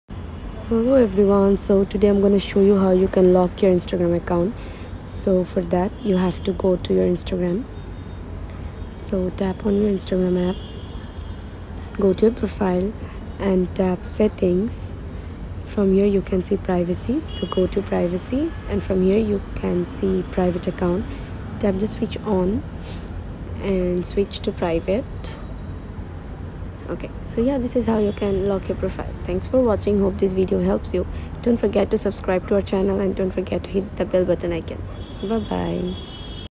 How to Lock Your Instagram Account and Keep Your Photos Private 2025 [New Method]
With the rise of social media, it's important to keep your personal information and photos private. One way to do this on Instagram is by locking your account. In this video, we'll show you how to lock your Instagram account and keep your photos and information safe from prying eyes. We'll go through the step-by-step process of enabling privacy settings on your account, including setting your profile to private and approving followers. Whether you're new to Instagram or an experienced user, this tutorial will help you keep your personal life private.